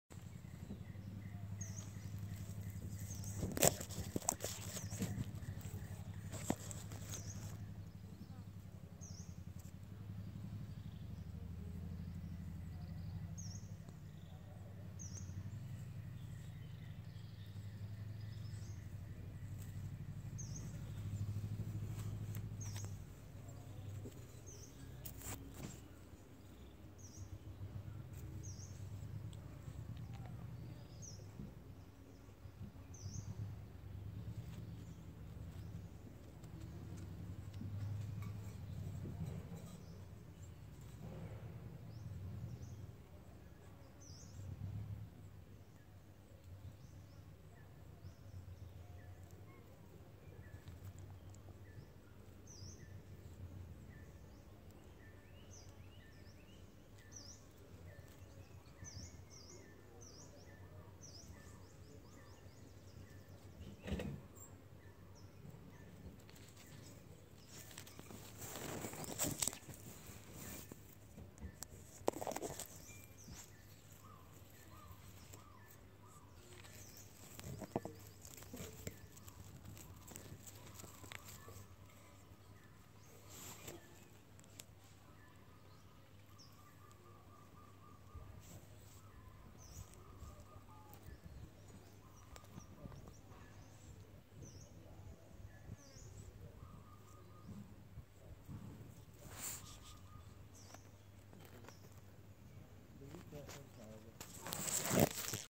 Close-up of otters - waiting for rescued
He is ready to be rescued, Forest ranger was taking him from the illegal wildlife market to a national park where he could have freedom on the river. God bless him